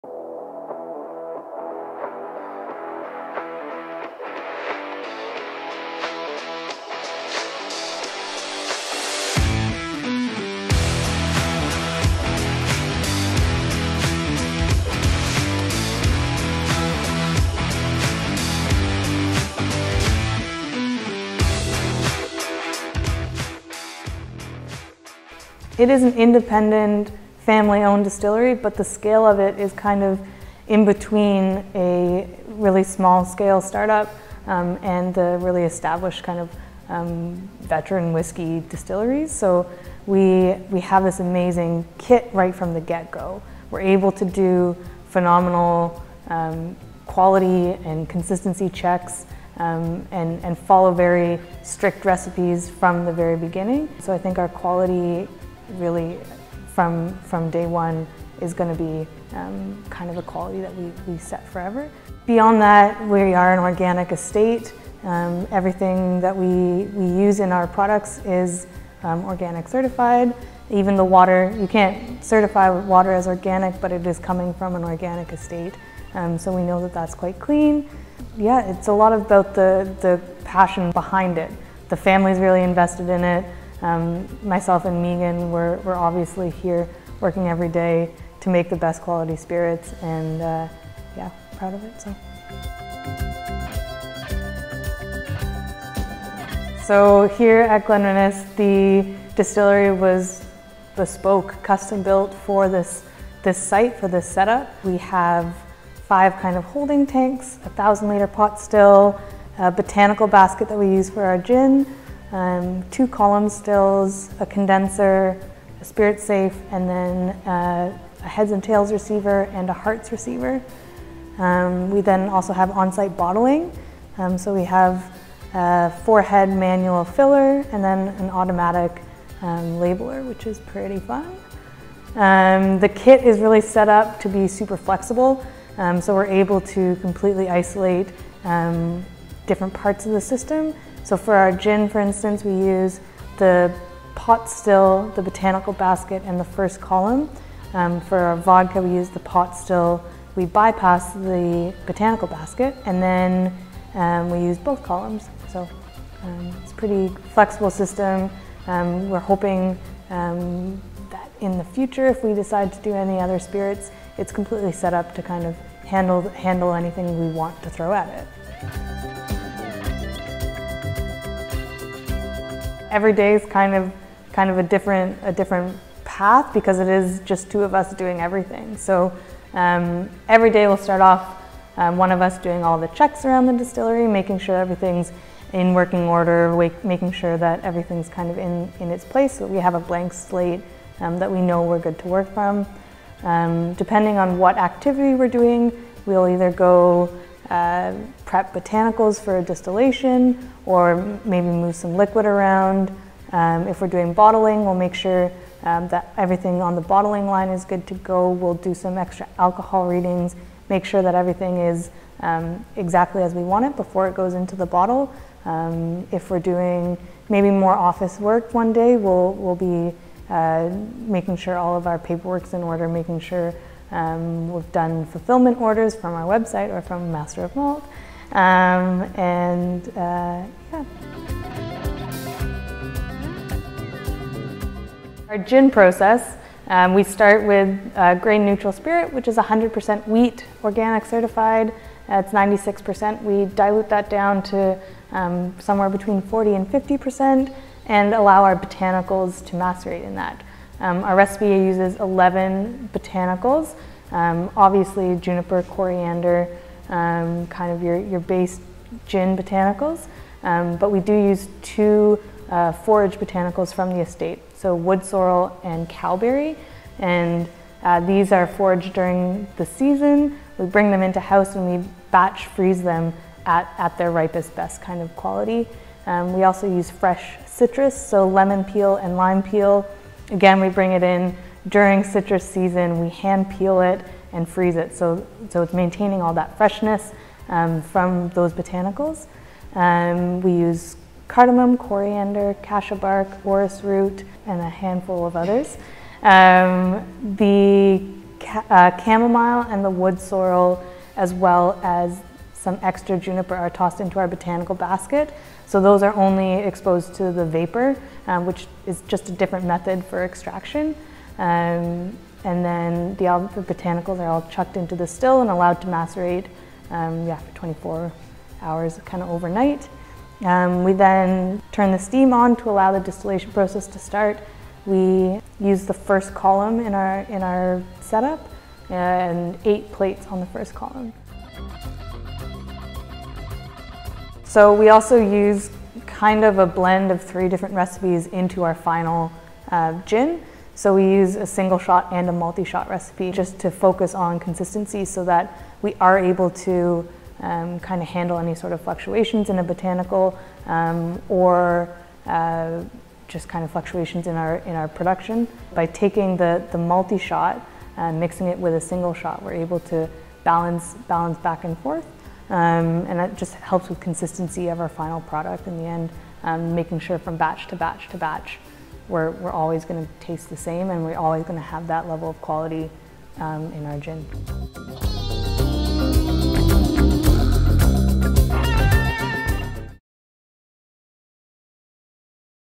Meet Katrina Stewart, distiller at Glenrinnes Distillery!
We chat to Katrina Stewart, distiller at Glenrinnes Distillery, to discuss its set-up, production of gin and vodka, and the family values of the company.